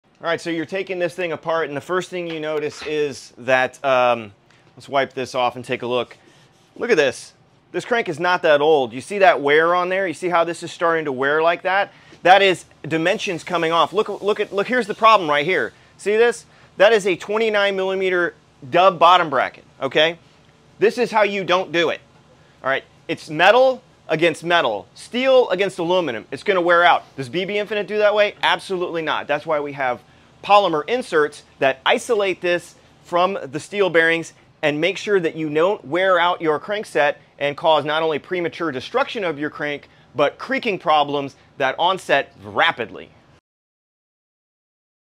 Your Bottom Bracket is Wearing Your Crankset/Chain Set Out
Metal on metal contact can be okay, but not when it's steel directly in contact with aluminum in a slip-fit application. There will be movement. Aluminum loses.